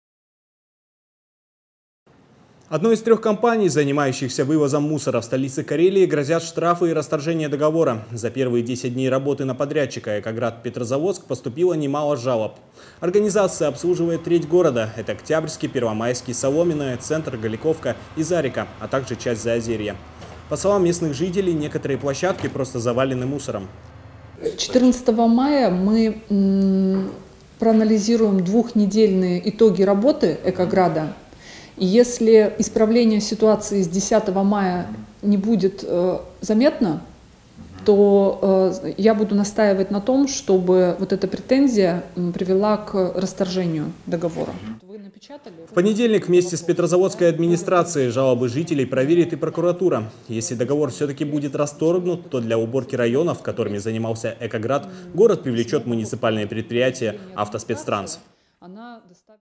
Не успевают убирать мусор
Одной из трех компаний, занимающихся вывозом мусора в столице Карелии, грозят штрафы и расторжение договора. За первые десять дней работы на подрядчика «Экоград-Петрозаводск» поступило немало жалоб.